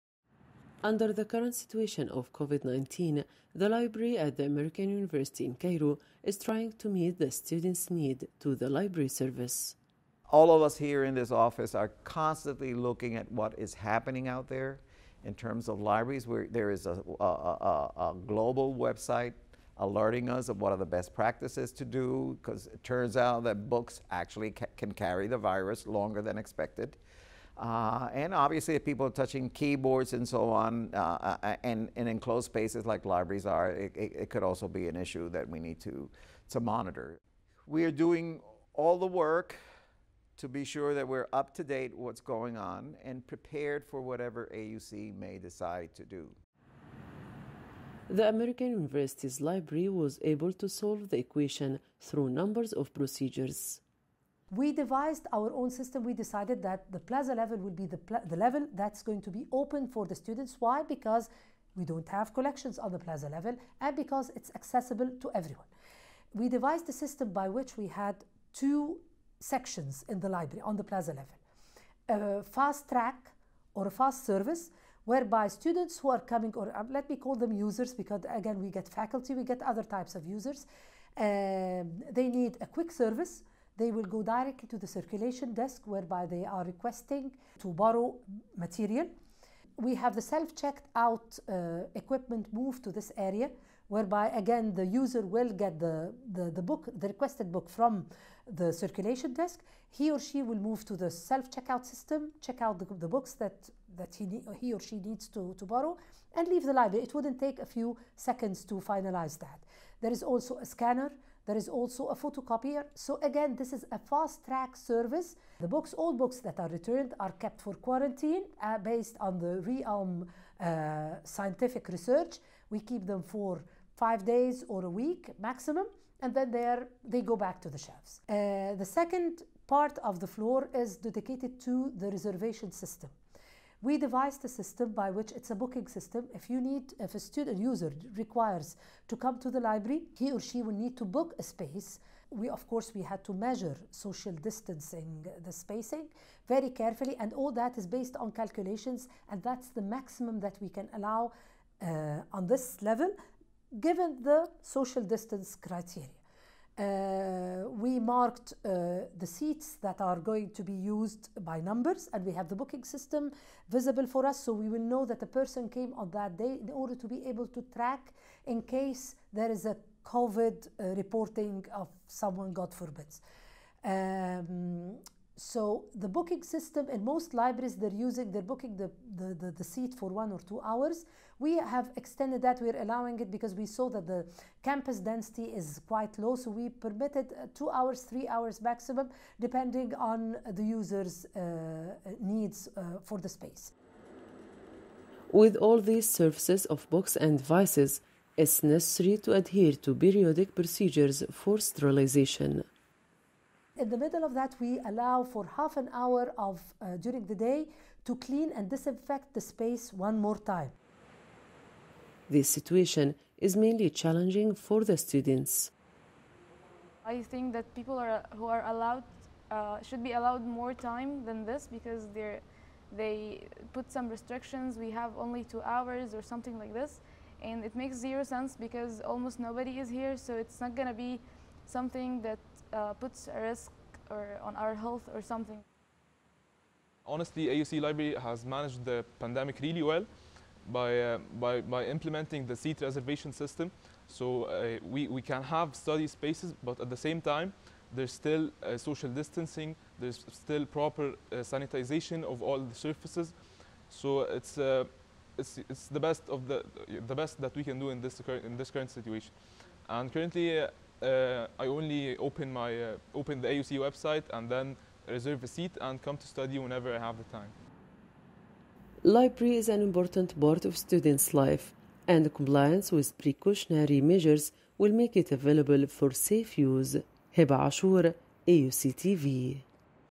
AUC Library During the COVID-19 pandemic
As most educational places, The library at AUC faces many challenges in the current situation of COVID-19, how do they confront it ? That's what we will know in the upcoming report with Heba Ashor.

مثل معظم الأماكن التعليمية ، تواجه المكتبة في الجامعة الأمريكية بالقاهرة العديد من التحديات في الوضع الحالي لفيروس كوفيد 19 ، كيف يواجهونها؟  هذا ما سنعرفه في هذا التقرير  مع هبة عاشور.